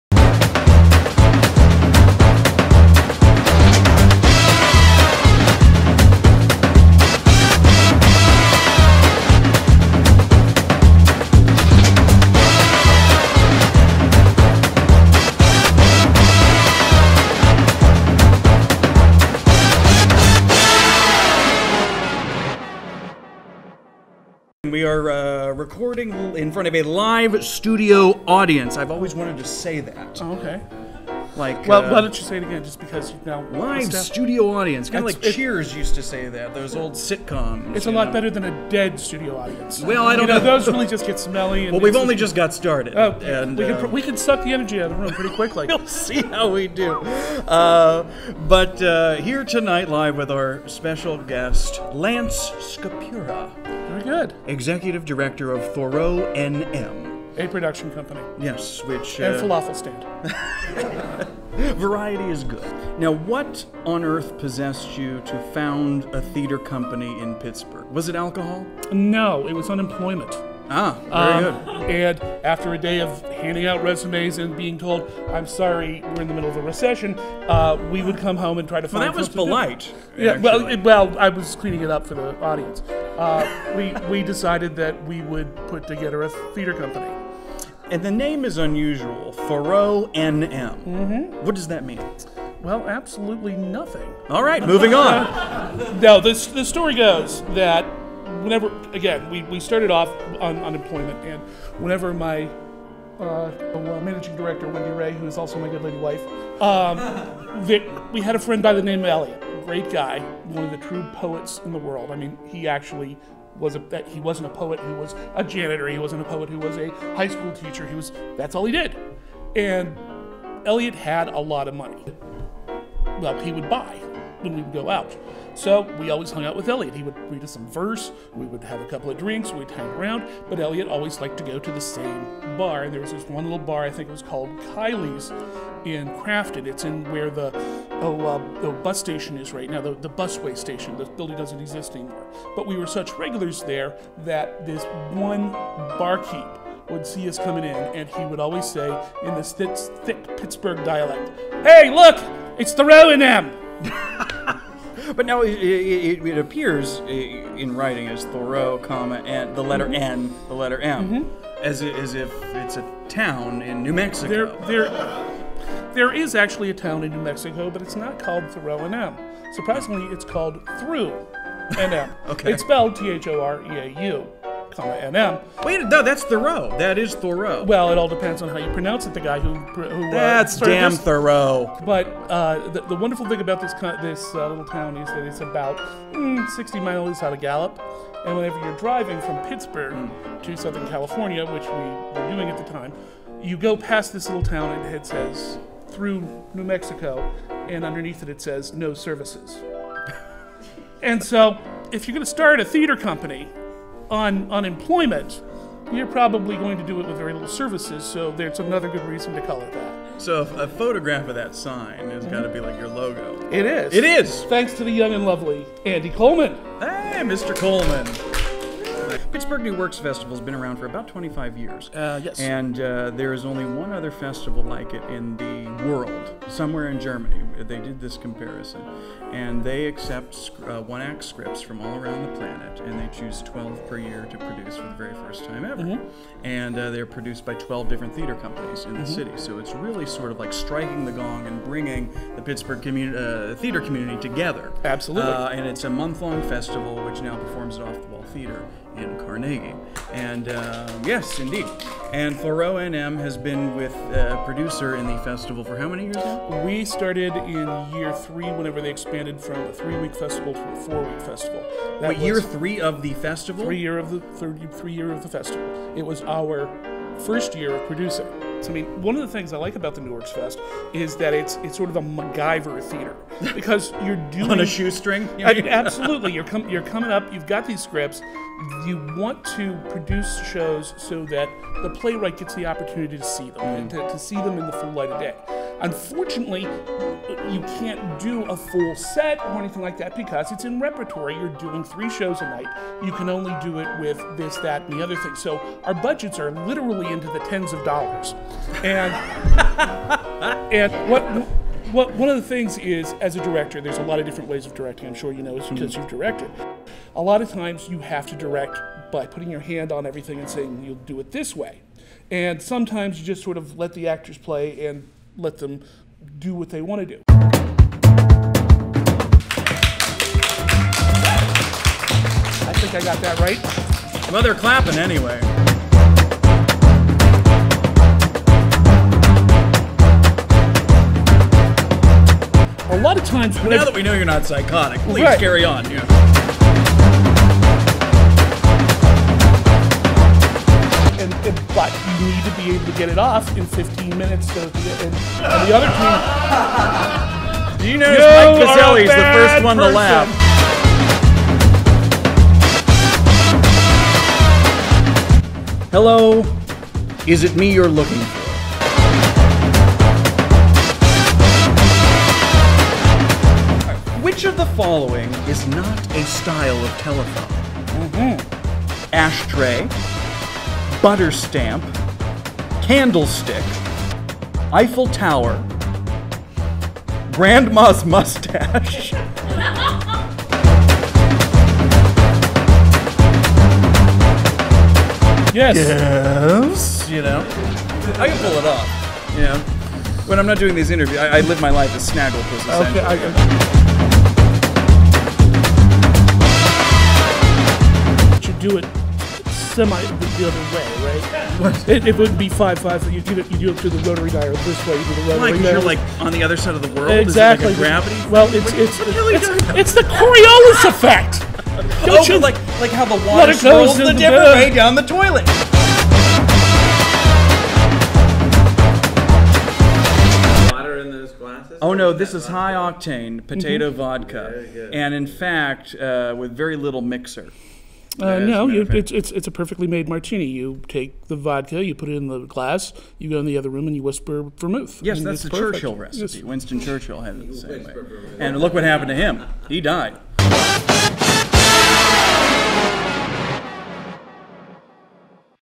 'BURGH VIVANT: LANCE SKAPURA - Executive Director, Thoreau NM
Filmed before a live audience at Arcade Comedy Theatre in beautiful downtown Pittsburgh, host Brian Edward laughs it up with Thoreau, MN Executive Director Lance Eric Skapura.  Through the course of a dry martini, Lance discusses the origins of Thoreau, its unusual name, and the company's long-standing involvement in the Pittsburgh New Works Festival.  Listen to "The Full Martini" - the complete unedited interview in audio podcast to hear Lance's insight on Thoreau NM's encore production of HOTLINE (which performed at Arcade immediately following this interview), absurd telephone trivia, and just who is Emma Nutt?